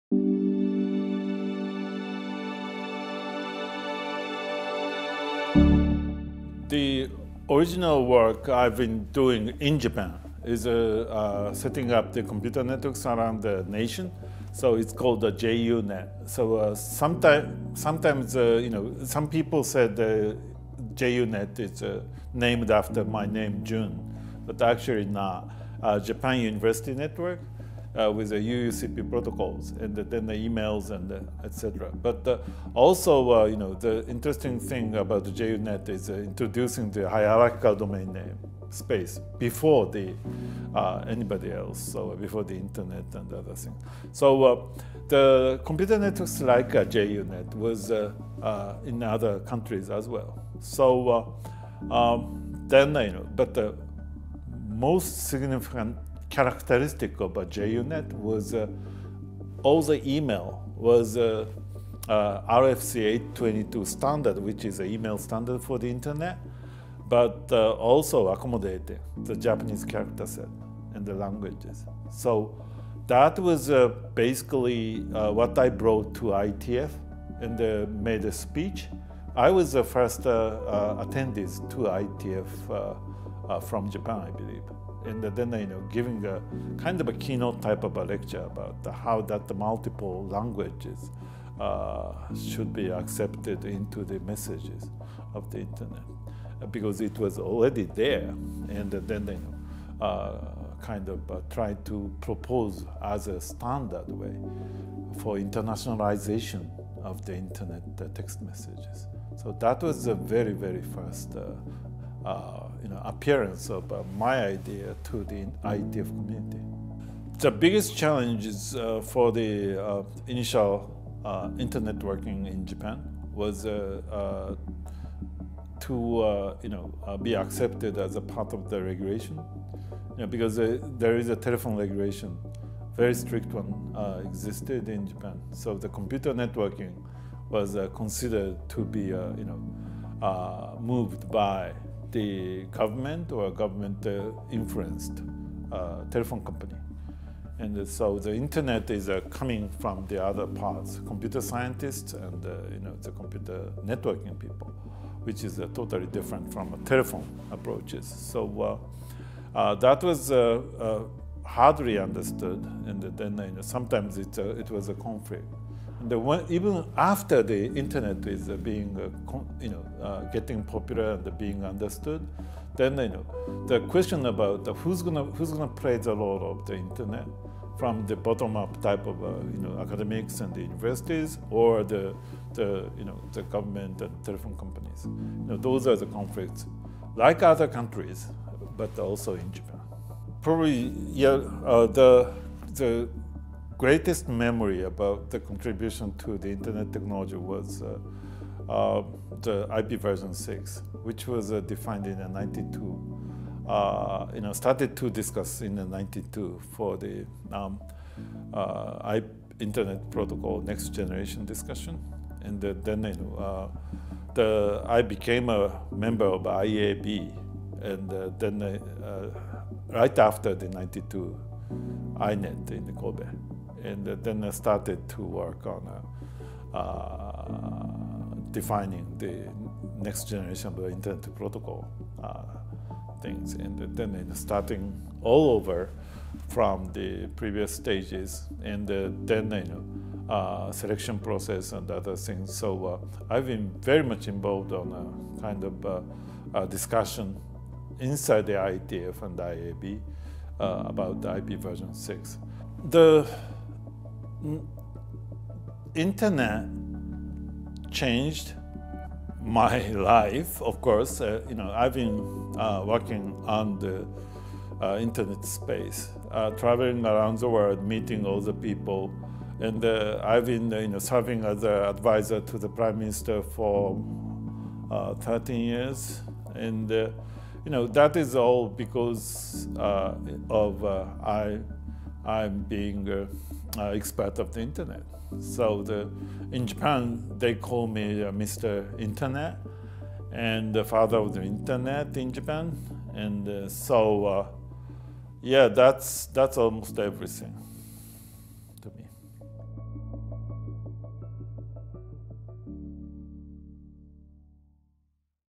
2013 Historic Profile - Jun Murai - Internet Hall of Fame Pioneer
Dr. Jun Murai is known as the 'father of the Internet in Japan,' and, outside of Japan, has also been called the 'Internet samurai.' In 1984, he developed the Japan University UNIX Network (JUNET), the first-ever inter-university network in that nation. In 1988, he founded the Widely Integrated Distributed Environment Project, a Japanese Internet research consortium, for which he continues to serve as a board member. He is a professor and dean of the Faculty of Environment and Information Studies at Keio University, from which he earned his PhD in computer science in 1987. He served as President of the Japan Network Information Center (JPNIC) and as Vice President of the Japanese Internet Association. He served on the Internet Society's Board of Trustees from 1997 to 2000. In 1998, he was appointed as one of nine initial directors of the Internet Corporation for Assigned Names and Numbers and served until June 2003. He won the 2005 Internet Society's Jonathan B. Postel Service Award in recognition of his vision and pioneering work that helped spread the Internet across the Asia Pacific region. He was honored with the 2011 Institute of Electrical and Electronics Engineers Internet Award.